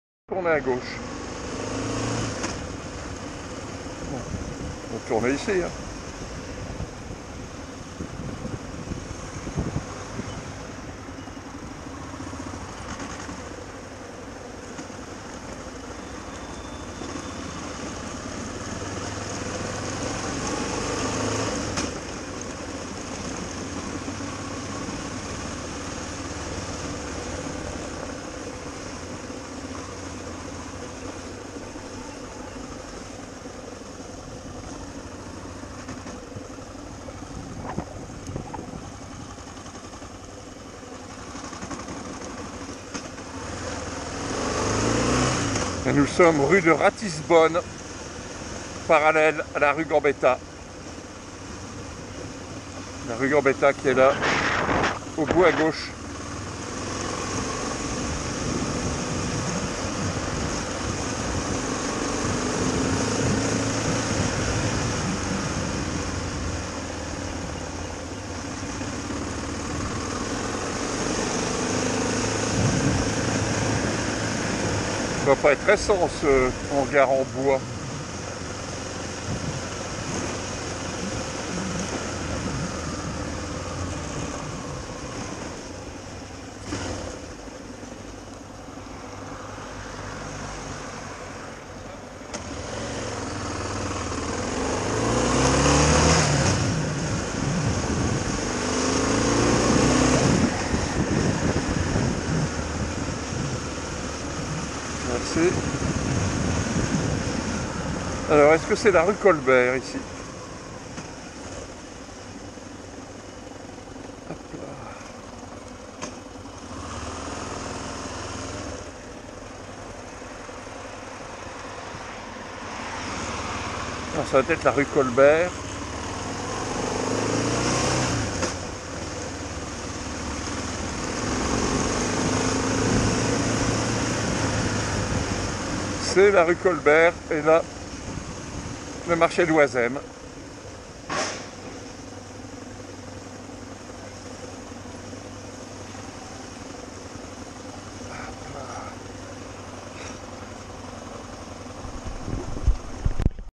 PROMENADES EN YAMAHA 600 XT DANS LILLE et ENVIRONS
8 Novembre 2019,
Lille,
Wazemmes,
Rue de Ratisbonne, Rue Jules Lefebvre, Rue Colbert.